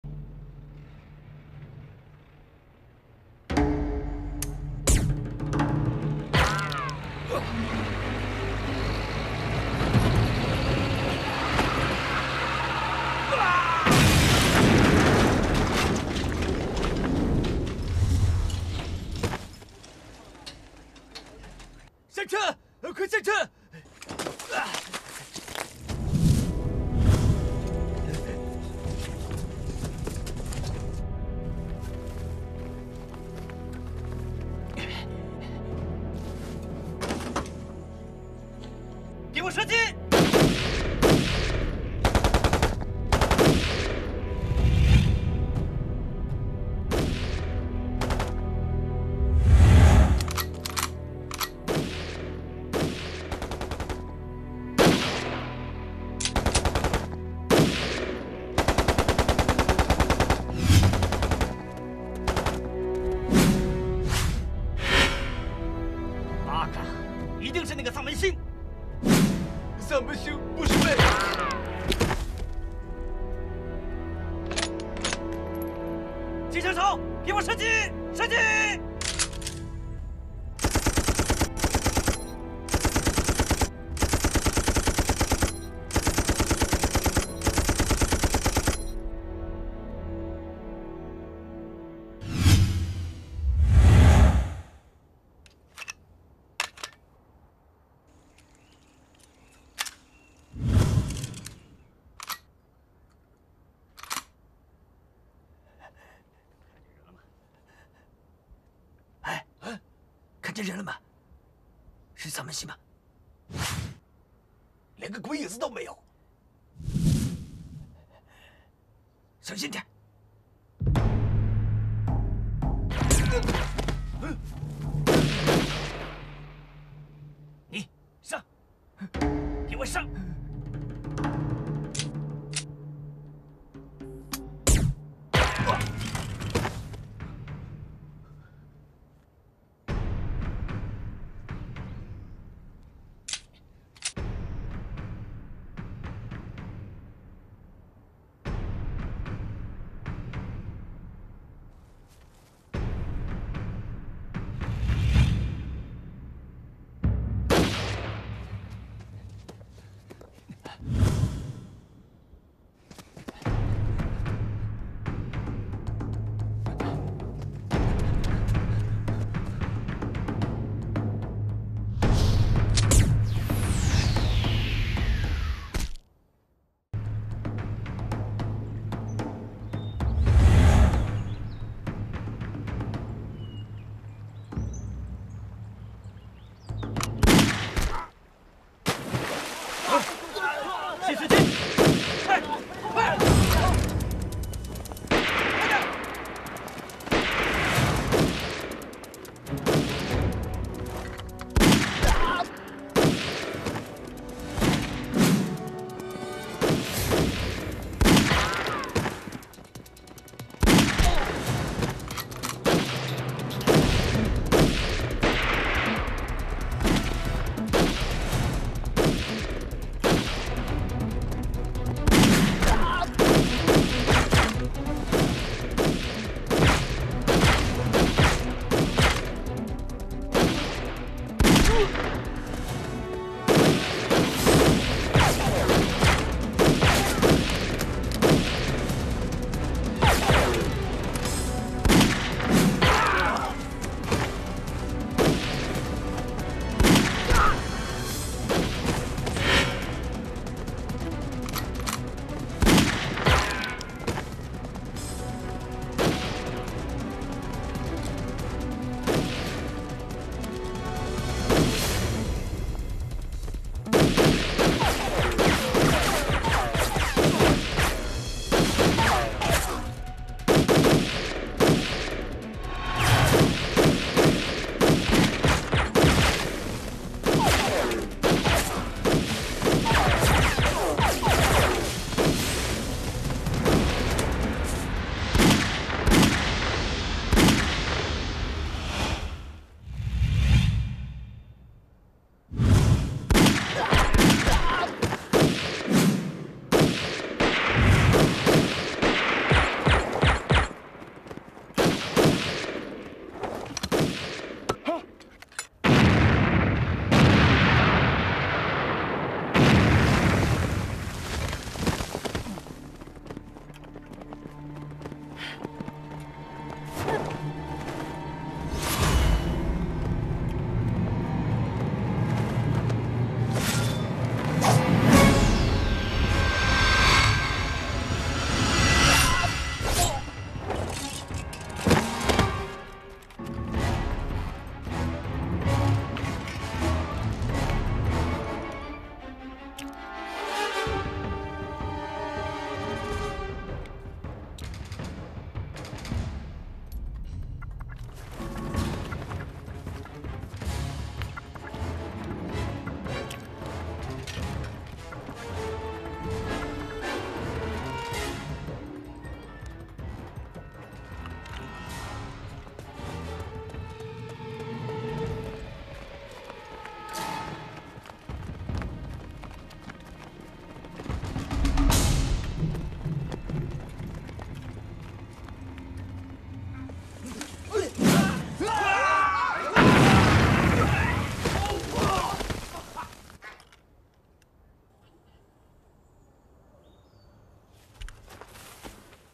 神槍小伙埋伏在草叢裡，一把消音狙架住了卡車裡的日軍！
sq